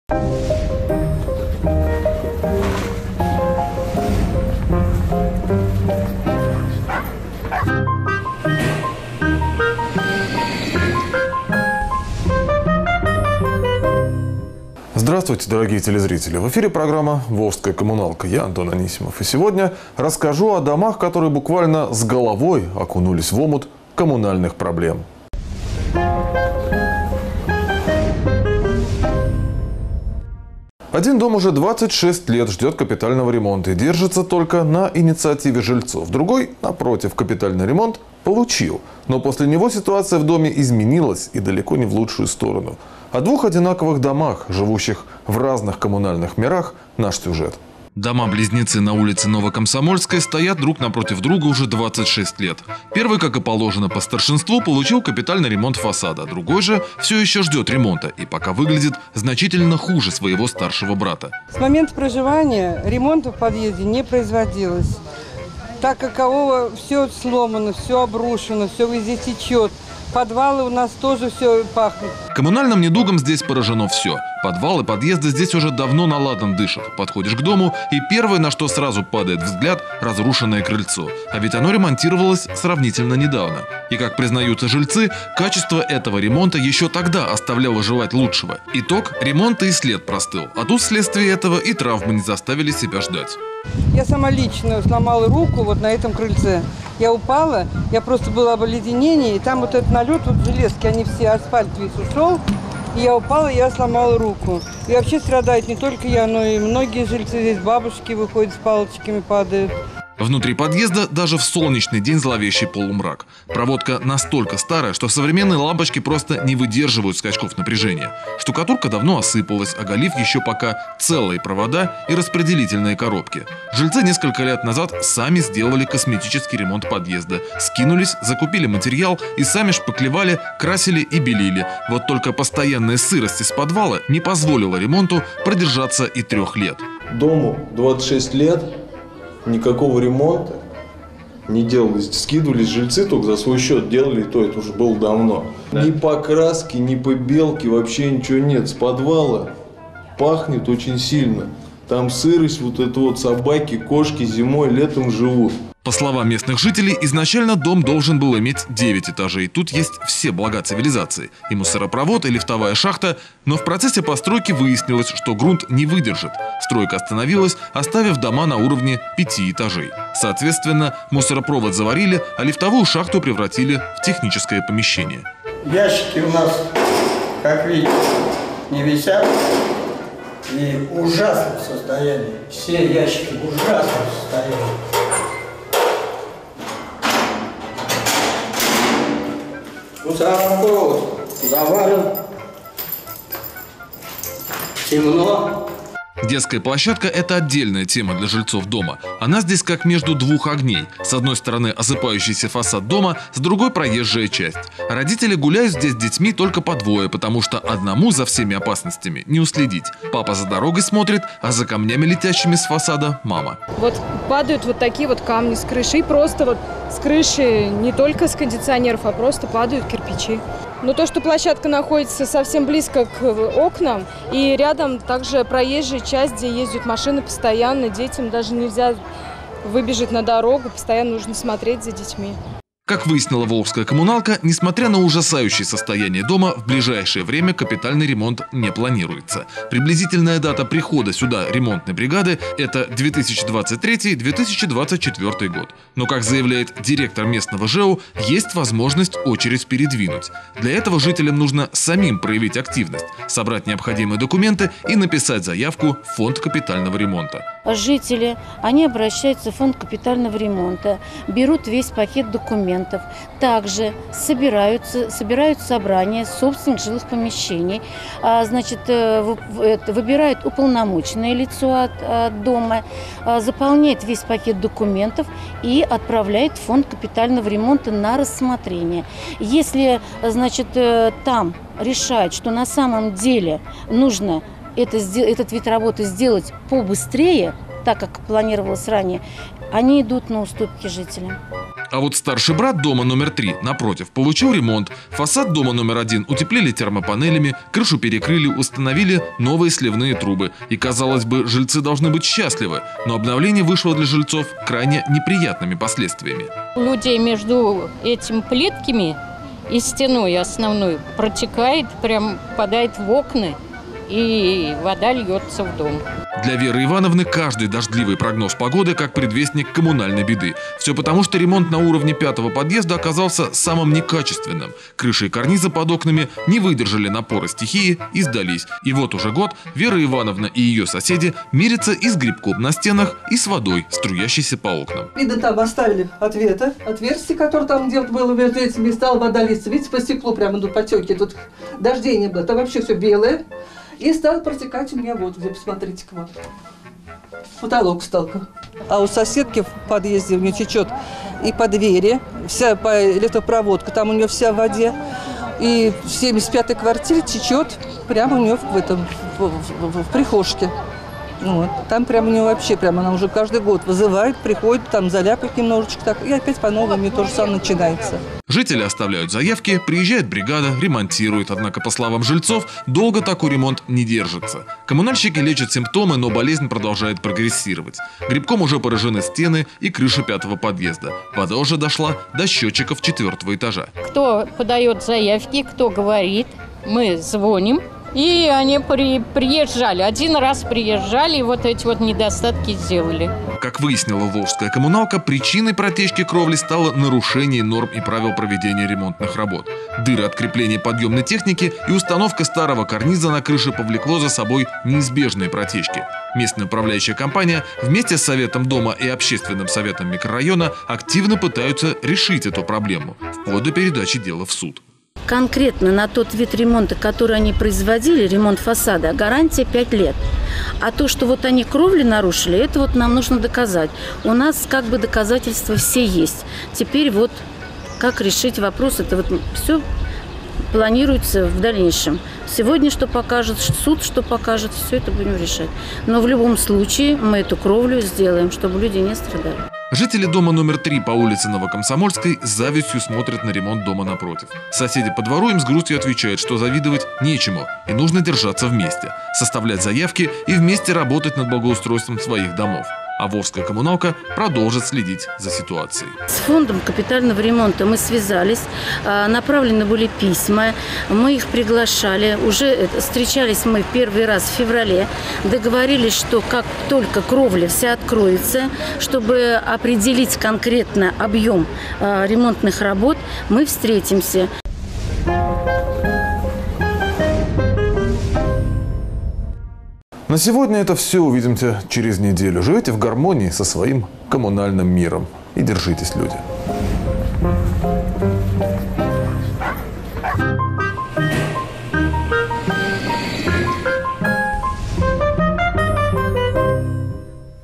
Волжская коммуналка 2.07.2015
Один дом уже 26 лет ждет капитального ремонта и держится только на инициативе жильцов. Другой - напротив, капремонт получил. Ситуация в доме изменилась и отнюдь не в лучшую сторону. О двух одинаковых домах, живущих в разных коммунальных мирах, смотрите наш сюжет.